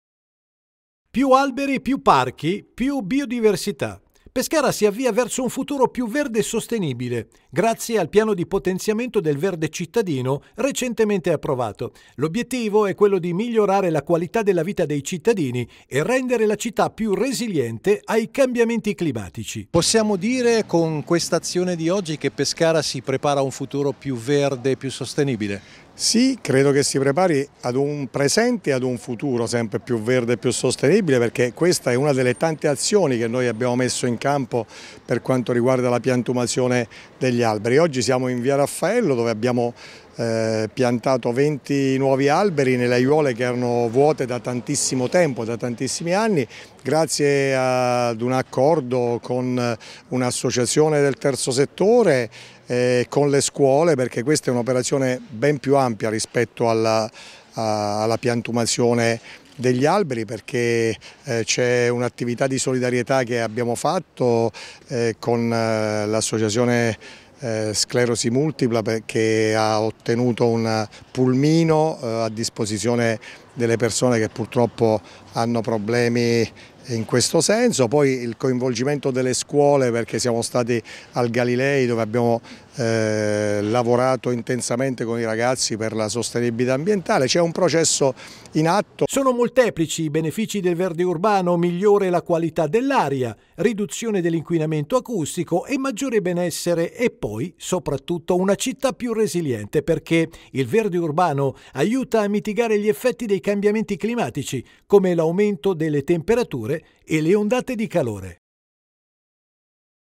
Pescara, una città più verde
Pescara si avvia verso un futuro più verde e sostenibile grazie al piano di potenziamento del verde cittadino recentemente approvato